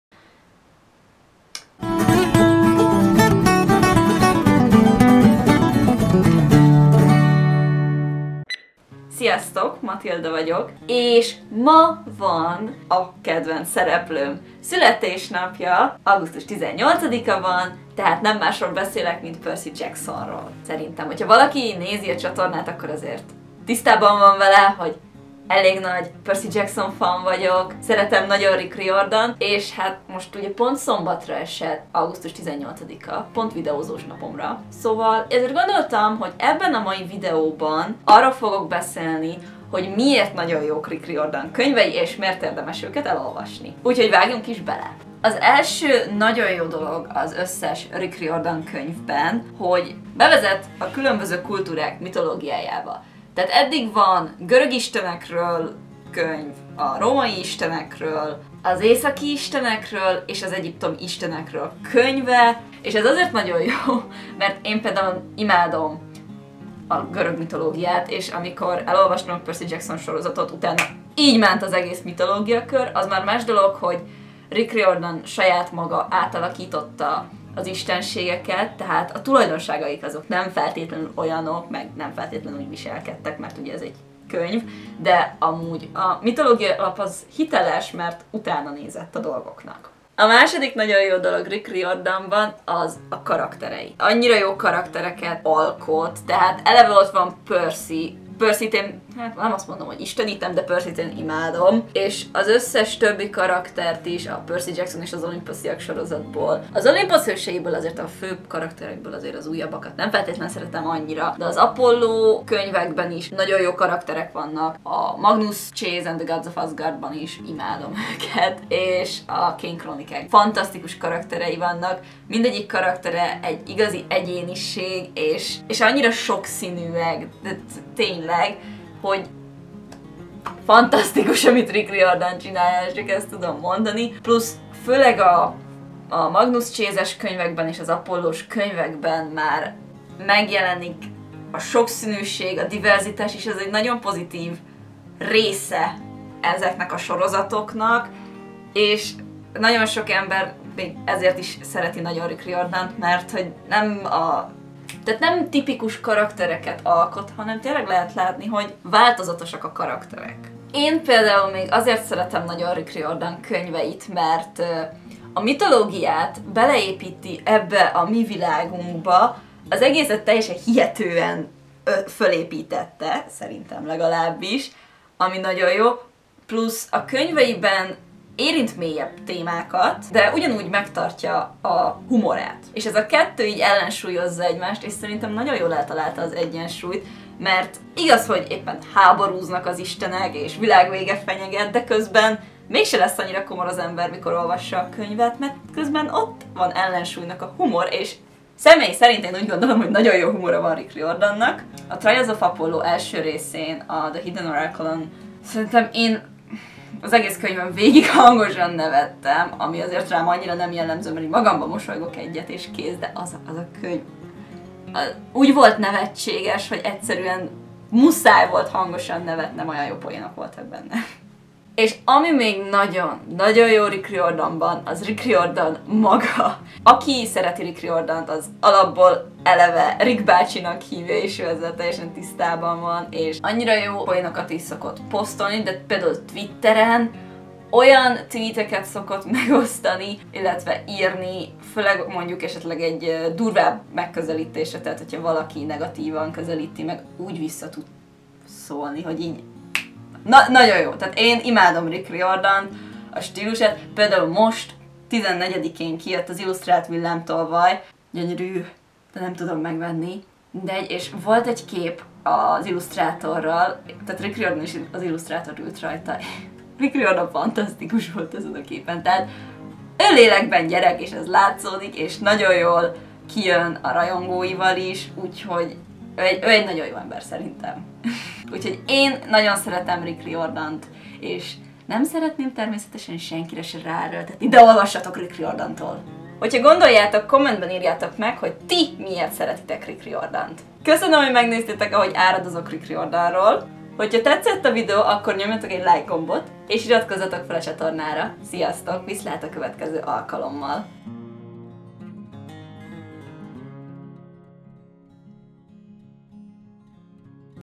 Boldog szülinapot, Percy! | Miért olvass Rick Riordant?
Ma van Percy születésnapja, és ennek ünnepére összeszedtem néhány érvet, hogy miért szeretek Rick Riordant olvasni. :)

Valaki megszámolja hányszor mondom ki Rick Riordan nevét? :D

Megjegyzés: Tisztában vagyok vele, hogy Rick Riordan nevét 'rájörden' ejtéssel mondják, de amikor magyarul beszélek, automatikusan úgy mondom, ahogyan le van írva. Ezért bocsánat, de így szoktam meg! :)